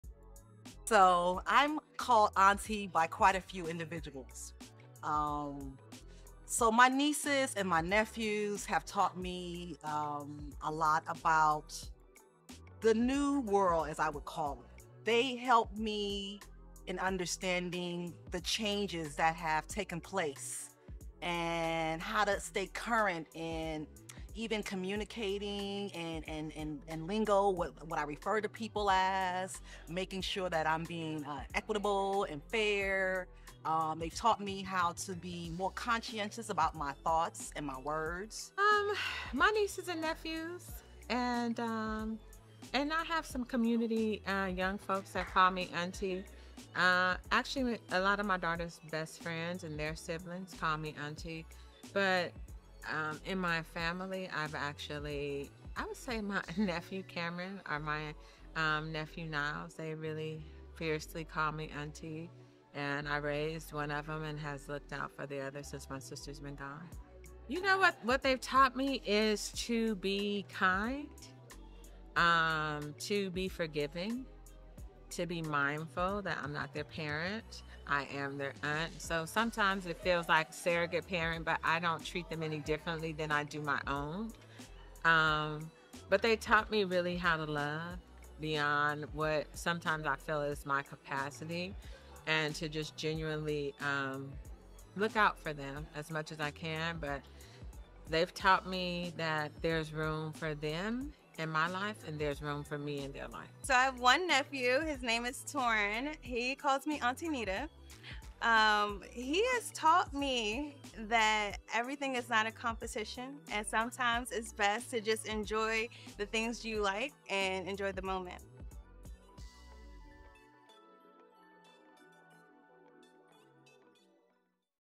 Fierce Aunties | Who Calls You Auntie and What Have They Taught You?
Who calls you auntie and what have they taught you?

We here at Black Women for Wellness believe that aunties should be celebrated and appreciated for all the ways you support, mother and invest in our communities every single day. We also believe that Aunties can play a very important role in helping to shape the future of the reproductive justice movement.

We know that the reproductive justice movement hasn’t always engaged our aunties in the conversation. We want to change that. We are inviting all Fierce Aunties to join us in the movement to secure safe and equitable sexual health services, abortions and reproductive health care for our loved ones.

Reproductive justice includes a full range of healthcare services from fertility treatments to equitable perinatal care; comprehensive sexual health education to safe healthy neighborhoods to raise our children and family planning.

The Reproductive Justice movement is so much more than abortion rights. This movement is interconnected with voter rights, environmental justice, employment rights, LGBTQIA rights—Reproductive Justice is a human right.

The Reproductive Justice movement centers Black women as the closest to the issue in securing health equity for women. Black Women for Wellness is calling on our Aunties to help us to build and sustain the movement.

We know Aunties are a support system for our moms, their sisters, they nurture the women in their lives from the grannies to the mamas to the babies. Come join us to learn more about how you can help further the Reproductive Justice Movement.